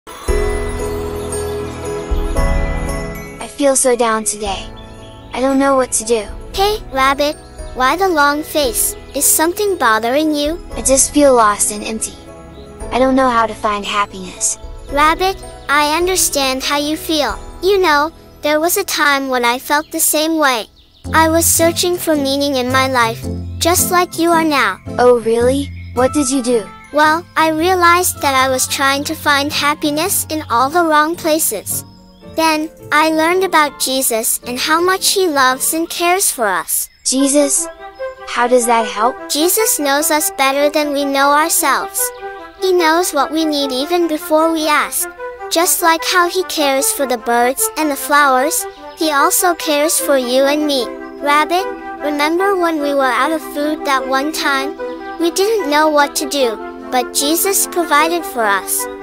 "Cute Little Friends"
In this heartwarming short animation series "Cute Little Friends", we meet two adorable characters - a turtle and a rabbit. The rabbit is feeling down and discouraged, but the turtle reminds him of Jesus' love and care. Through their sweet conversation, we are reminded that no matter what we're going through, Jesus is always there for us with open arms. This short is sure to leave you feeling uplifted and inspired by the power of faith and friendship.